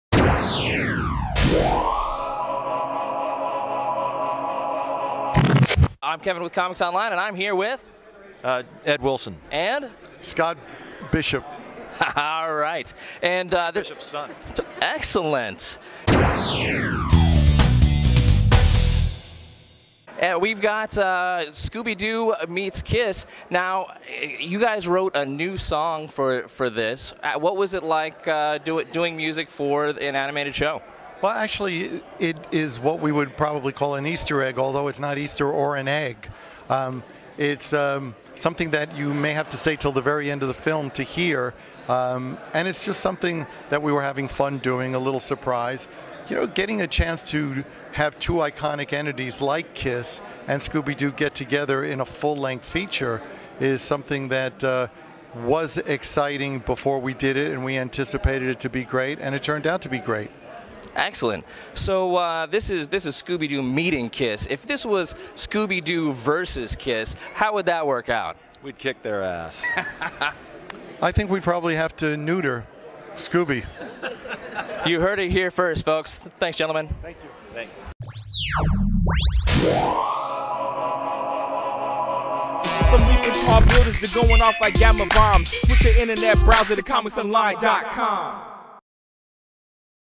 SDCC 2015 - Stanley and Thayer - Scooby Doo and Kiss
ComicsOnline's Editor-in-Chief Kevin Gaussoin interviews Kiss guitarists Paul Stanley and Tommy Thayer at San Diego Comic-Con 2015 about their roles on the new Scooby Doo animated feature: Scooby Doo and Kiss: Rock and Roll Mystery.